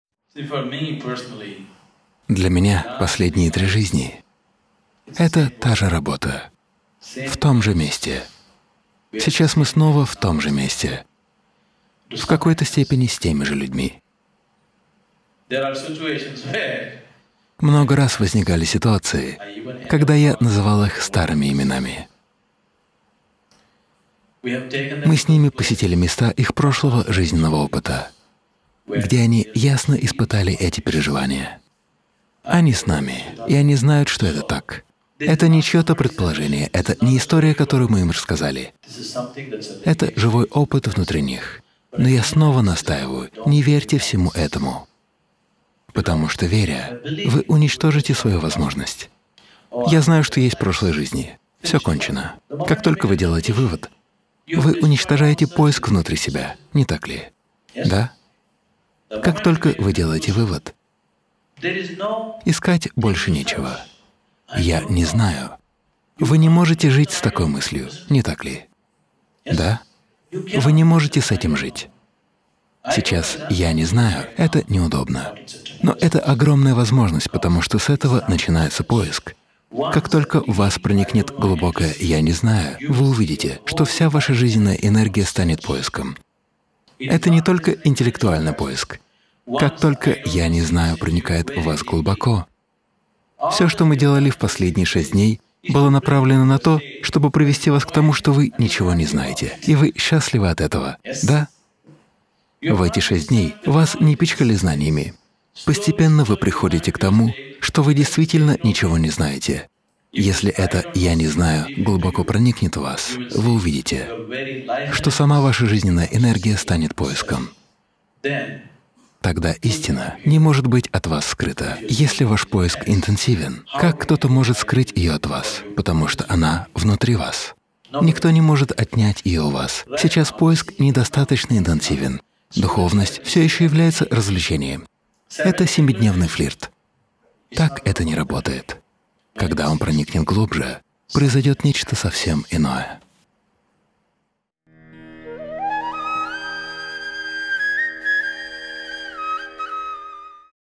С чего начинается настоящий духовный поиск?
Нужно ли вам знать о своих прошлых жизнях? В новом видео Садхгуру рассказывает о том, чем идея о реинкарнации может вам навредить, а также о том, как углубить свой духовный поиск.